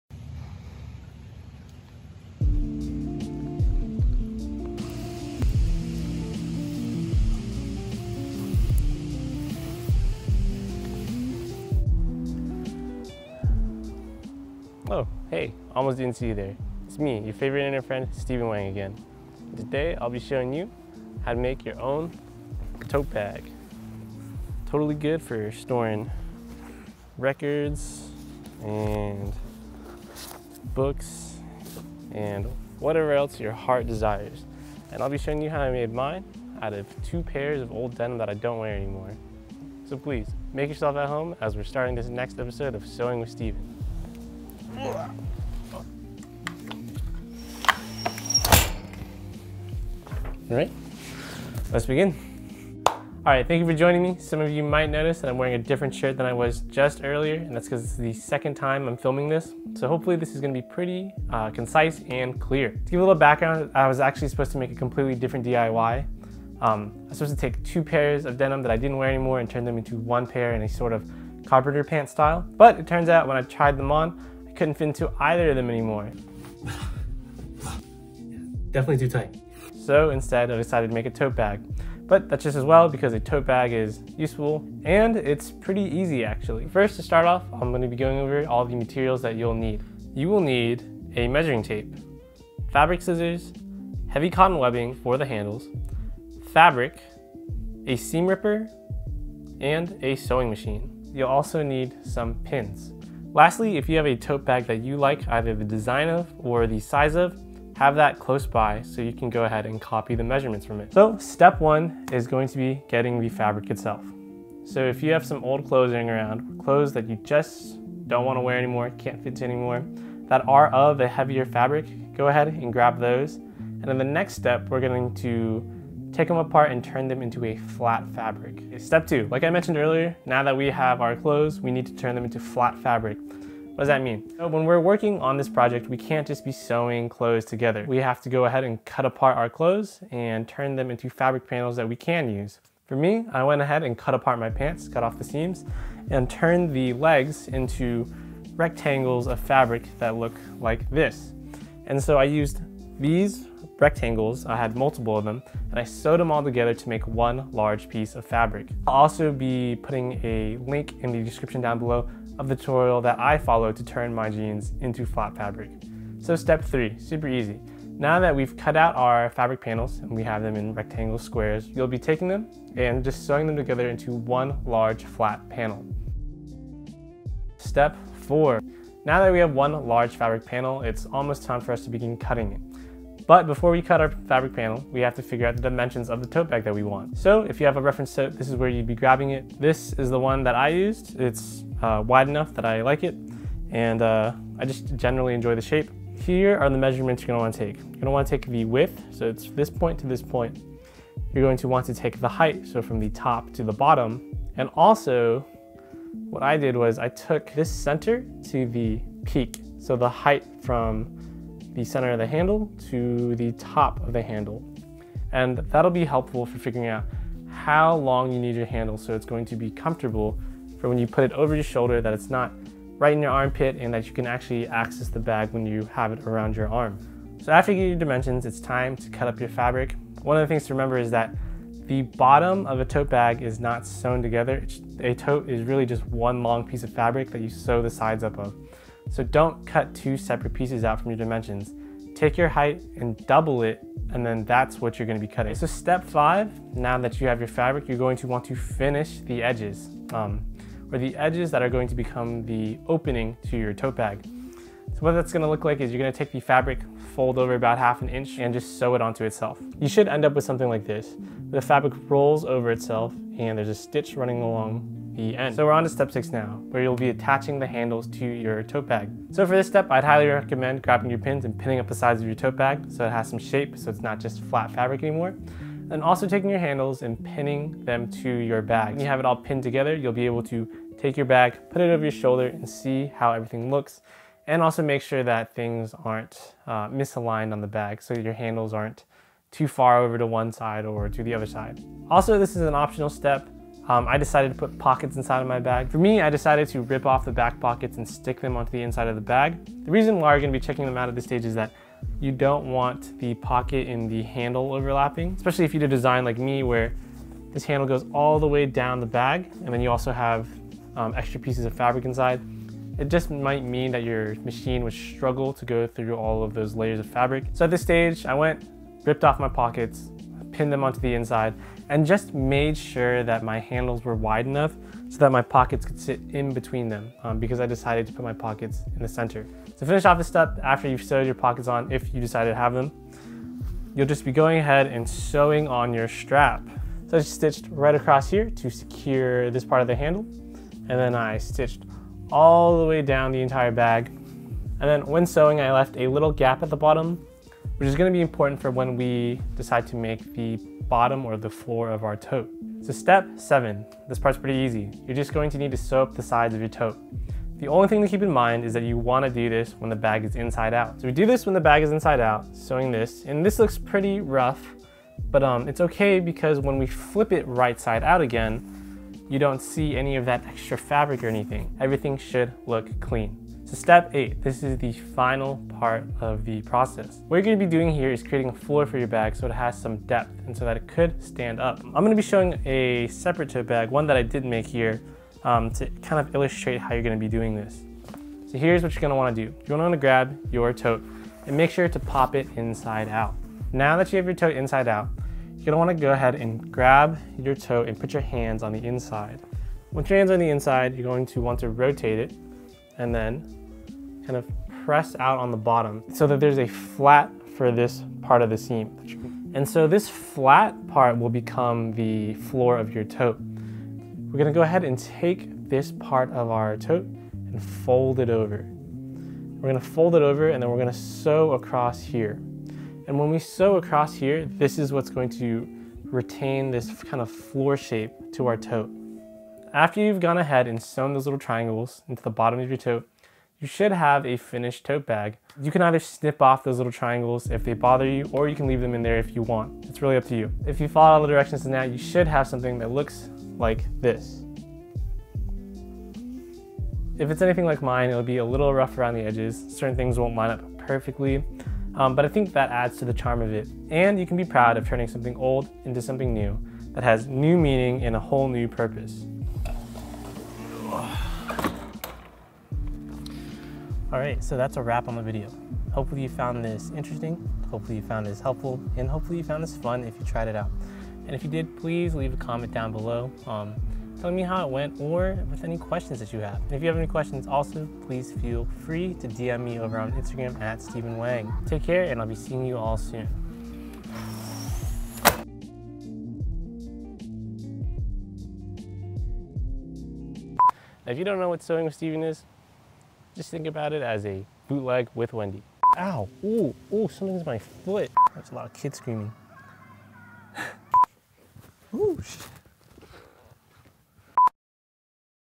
DIY Denim Tote | Sewing with Steven
Hello, this week I decided to make a tote bag after figuring out that my pants were too small for me to fit into (quarantine gains). Hopefully this is helpful, enjoy!